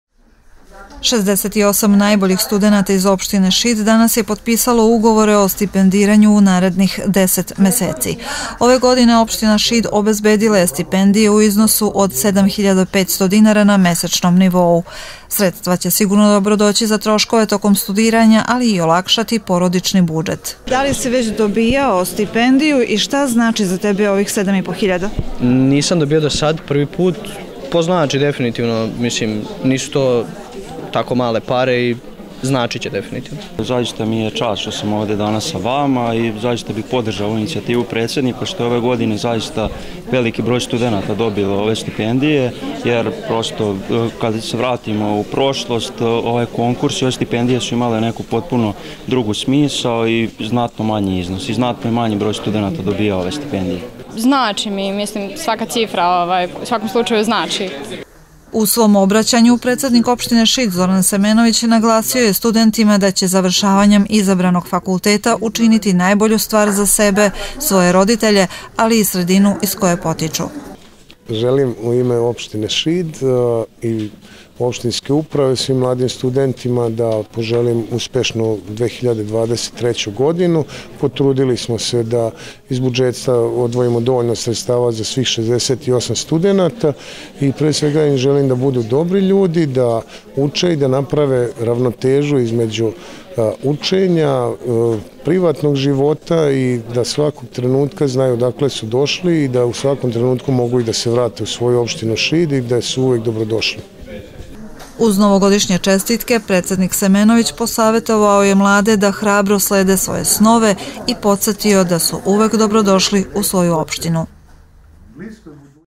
Dodela opštinskih stipendija
U sali skupštine opštine Šid danas je upriličeno potpisivanje i uručenje ugovora o dodeli studentskih stipendija za studente koji su po Javnom konkursu opštine Šid ostvarili uslove i stekli pravo na stipendiranje tokom akademske 2022/2023. godine. Prvi čovek lokalne samouprave Zoran Semenović studentima je poželeo srećnu Novu 2023. godinu i puno uspeha u daljem obrazovanju